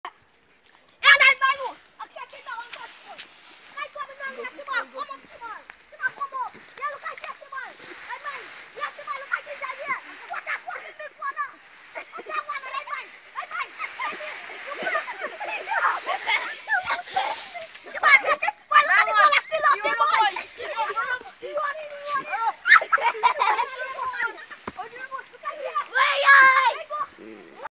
Find that lizard
Two boys try to catch a lizard for dinner